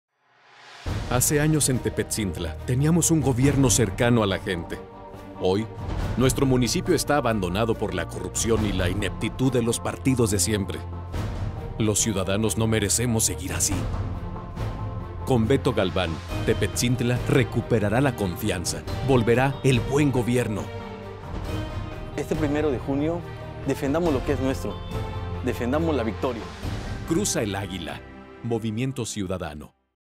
Beto Galván Tepetzintla - Movimiento Ciudadano.
Una vez más los políticos de siempre quieren adueñarse de Tepetzintla, vamos a defender lo que es nuestro, vamos a defender nuestro triunfo.

Tepetzintla regresará al buen gobierno.